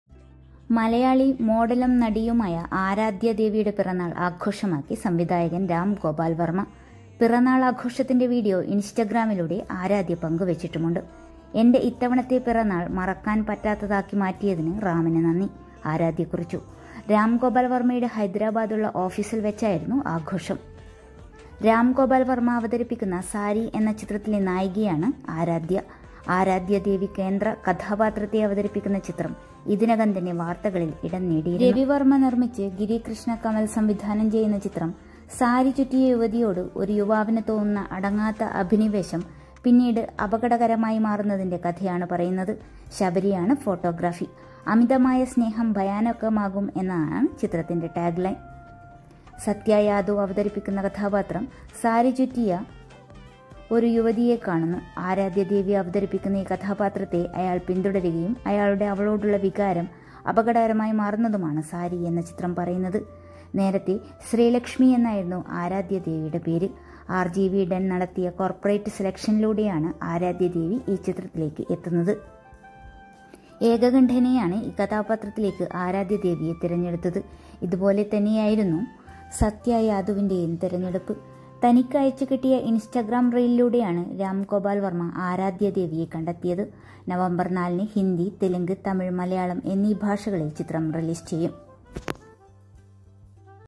ആരാധ്യയുടെ പിറന്നാള്‍ ആഘോഷമാക്കി രാം ഗോപാല്‍ വര്‍മ്മ; പിന്നാലെ സൈബര്‍ ആക്രമണം.aaradhyadevi birrhday
രാം ഗോപാൽ വർമയുടെ ഹൈദരാബാദുള്ള ഓഫിസിൽ വച്ച് അർദ്ധരാത്രി പന്ത്രണ്ട് മണിക്കായിരുന്നു ആഘോഷം.